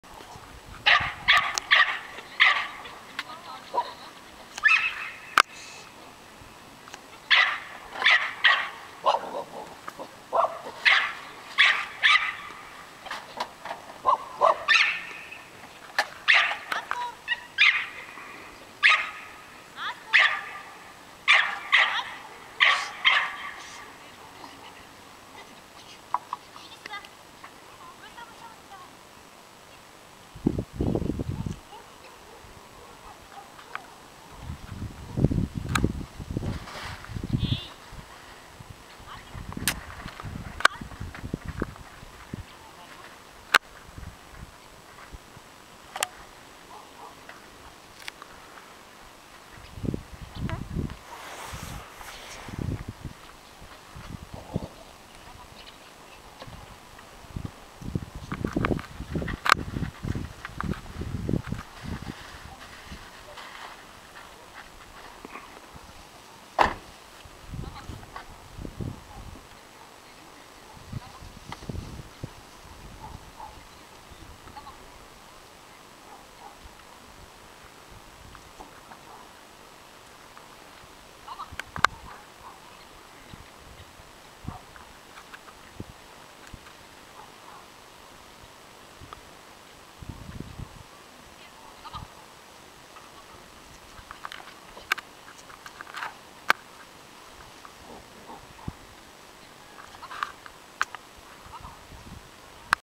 Naskur is herding sept/2008 third time -1
Naskur is herding sept/2008 third time -1 (with no trainer just doing what he wants)